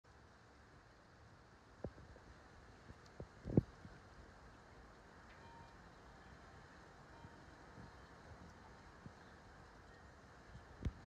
Hummingbird Backyard Visitor
Both our back and front yards are set up for pollinators and nectar drinkers so we see them often.